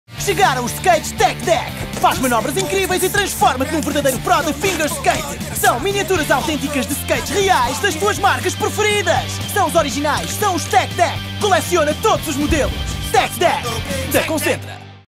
Tech Deck - Skates
Os Tech Deck estão de volta! São miniaturas autênticas de skates reais das tuas marcas preferidas! Monta os skates, aprende e replica os truques mais fixes, só precisas de imaginação! Leva-os para todo o lado e desafia os teus amigos para praticarem contigo. Coleciona todos e torna-te num pro em finger skate!!